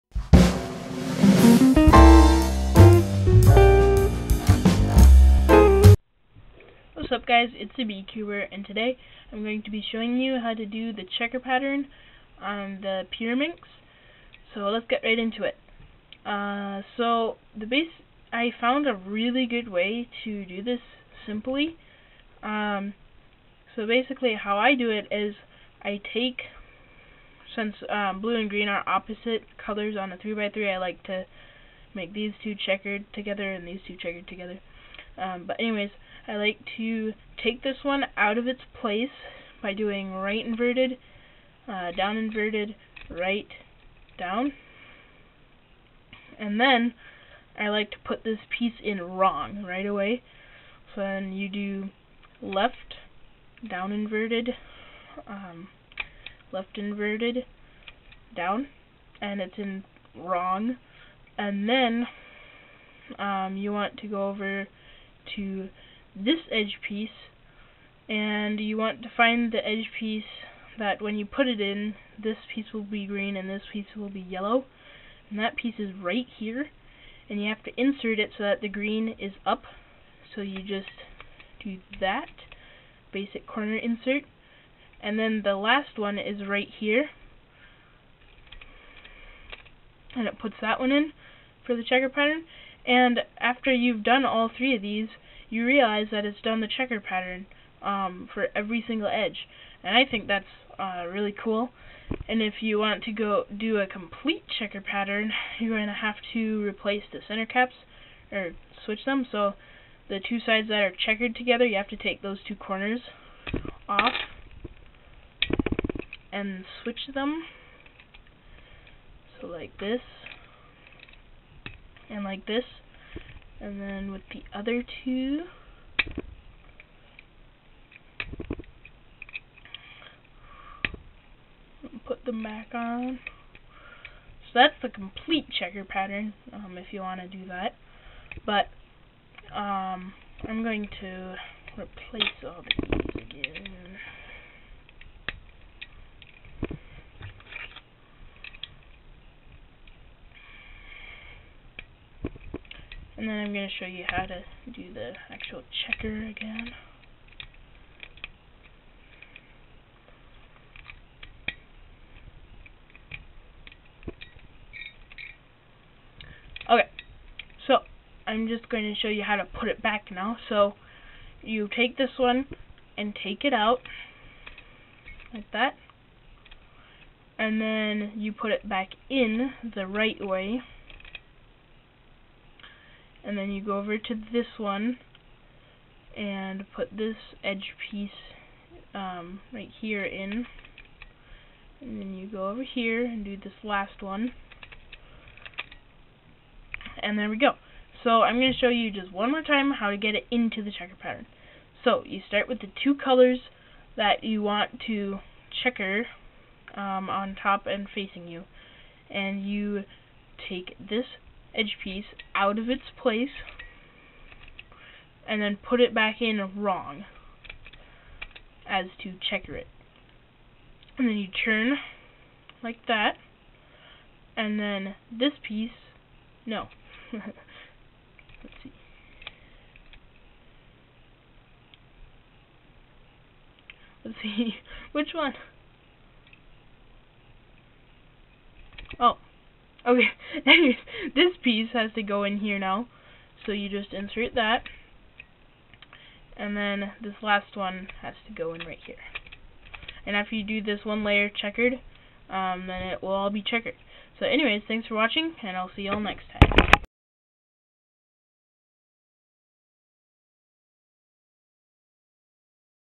How to Checker the Pyraminx!
Hey Guys! In this video I show you how to checker the pyraminx!